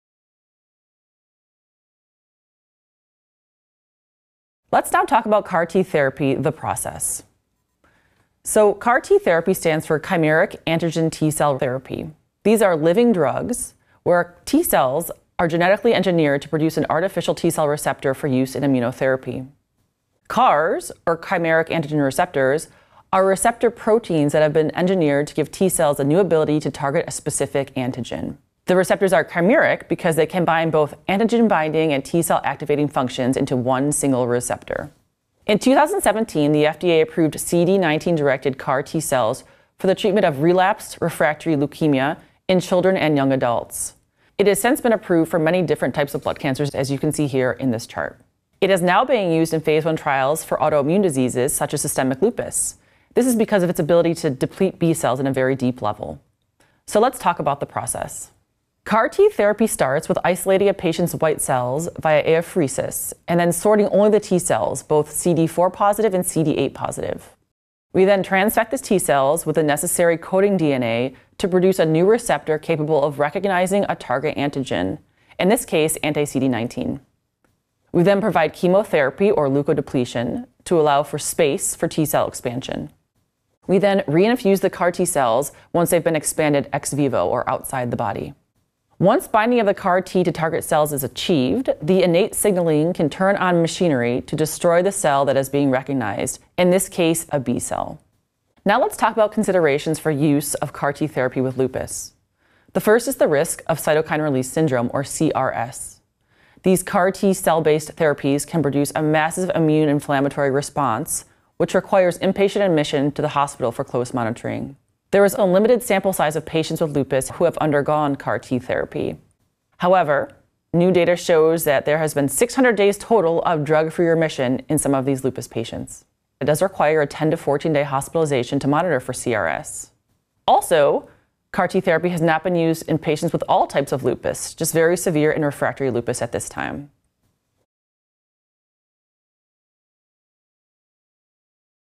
CAR T Cell Therapy for Lupus: The Process
Lupus specialist Emily Littlejohn, DO, MPH, explains the process of using CAR T Cell therapy for lupus.